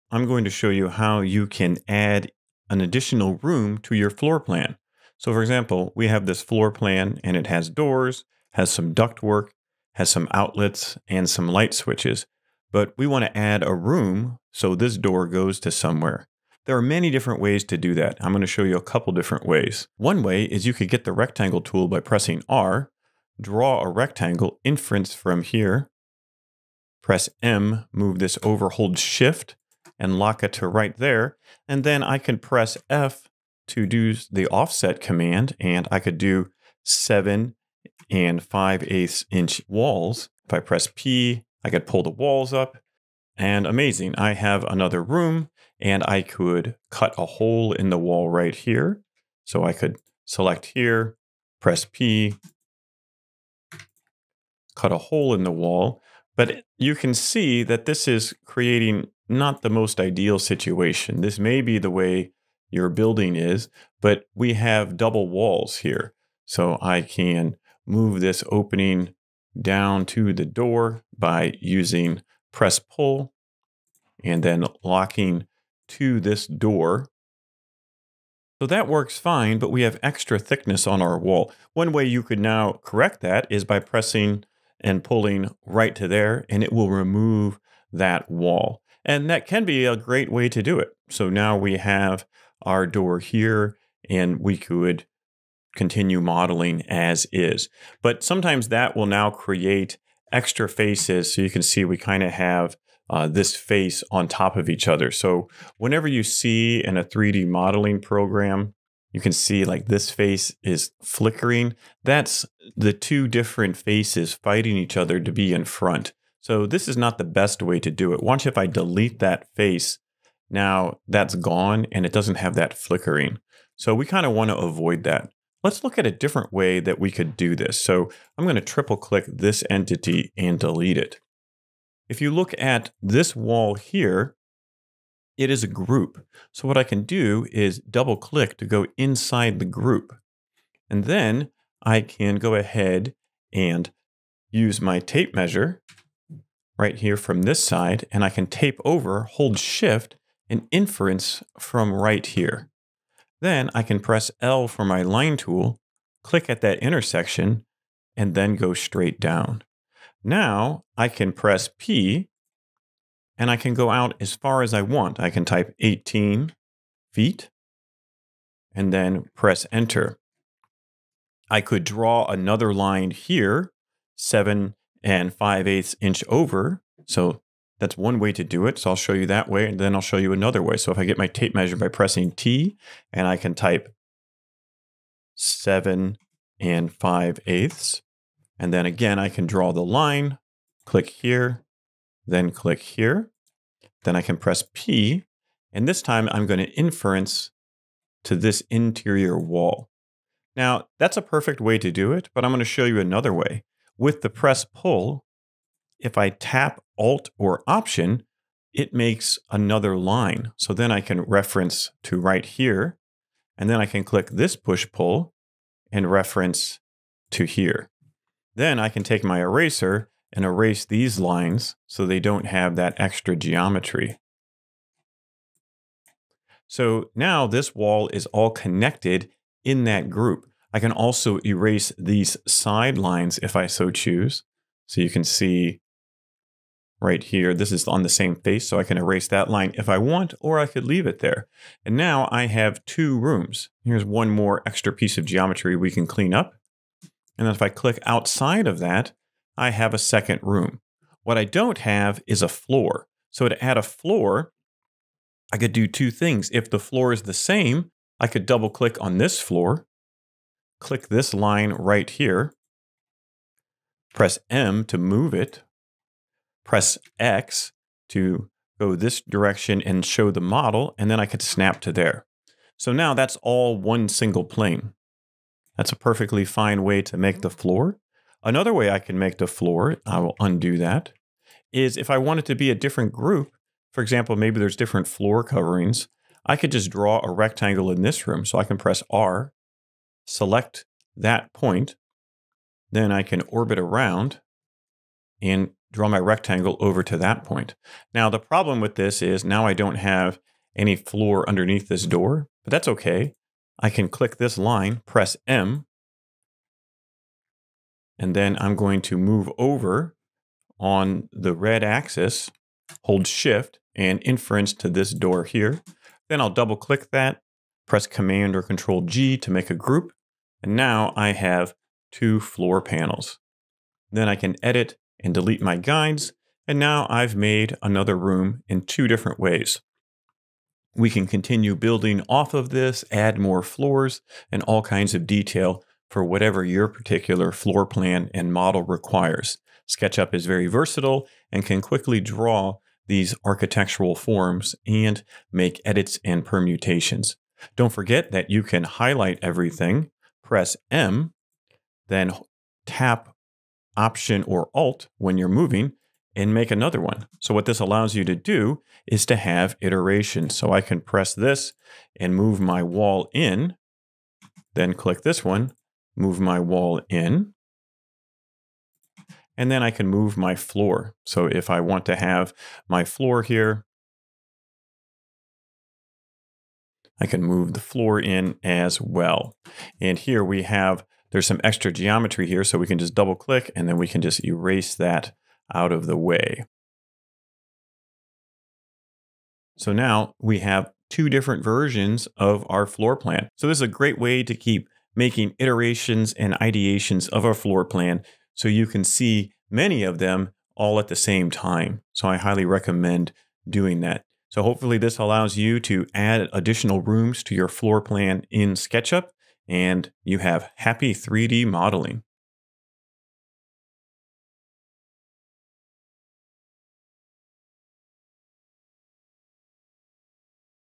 Floorplan in Sketchup Tutorial
How to Add Additional Rooms to Your Floor Plan in SketchUp

In this video, I will show you how to add additional rooms to your floor plan in SketchUp. I will demonstrate two different methods, and I will also provide tips on how to avoid creating extra faces or geometry.

Method 1: Using the Rectangle Tool

1. Open SketchUp and create a new file.
2. Click on the Rectangle Tool (R).
3. Draw a rectangle on the floor plan where you want to add a new room.
4. Use the Offset Tool (F) to create walls around the rectangle.
5. Use the Push/Pull Tool (P) to raise the walls to the desired height.

Method 2: Using the Tape Measure Tool

1. Open SketchUp and create a new file.
2. Click on the Tape Measure Tool (T).
3. Measure the width and length of the room you want to add.
4. Draw a line on the floor plan that is the same width and length as the room you want to add.
5. Use the Push/Pull Tool (P) to raise the line to the desired height.

Tips

* To avoid creating extra faces or geometry, make sure that you only create one wall for each room.
* You can use the Eraser Tool (E) to delete any extra faces or geometry that you create.
* You can use the Undo Tool (Ctrl+Z) to undo any mistakes that you make.

I hope this video helps you to add additional rooms to your floor plan in SketchUp. Happy modeling!

Links